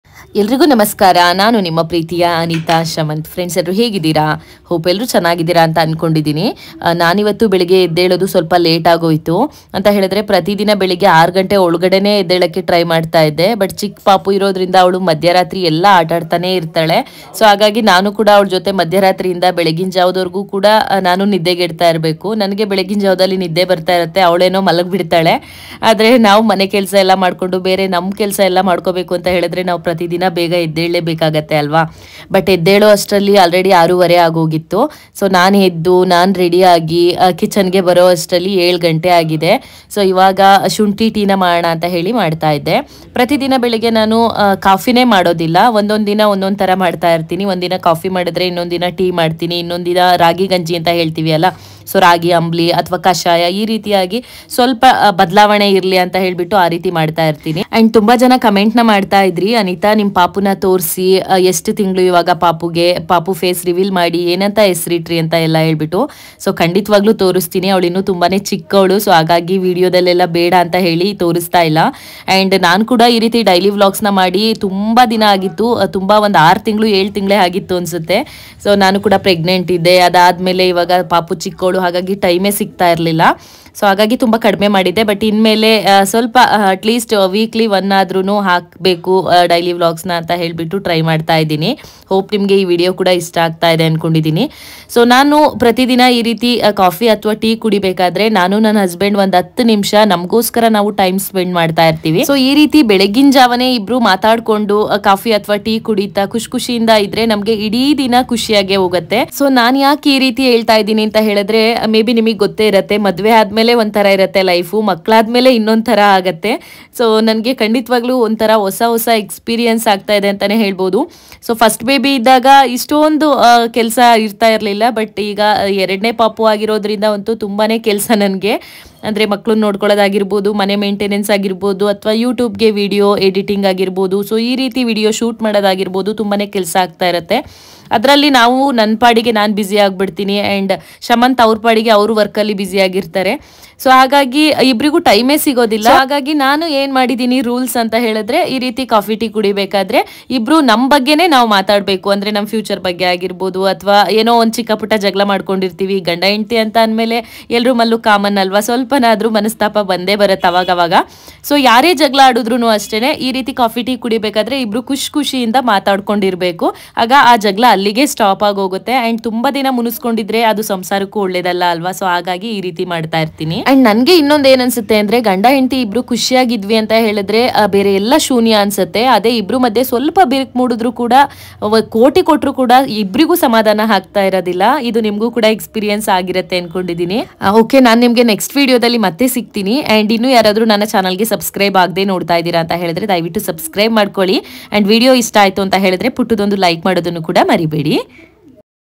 Indian housewife early morning vlog /ಶುಂಠಿ ಟೀ /ಅನಿತ ಶಮಂತ್
ಸಂಪ್ರದಾಯಕ ಮನೆತನದಲ್ಲಿ ಹುಟ್ಟಿ ಅಚ್ಚು ಮೆಚ್ಚಿನ ಗೃಹಿಣಿಯಾಗಿ,ಹೆಂಡತಿಯಾಗಿ,ತಾಯಿಯಾಗಿ ಇರುವ ಎಲ್ಲರಿಗೋಸ್ಕರ ಈ ವಿಡಿಯೋ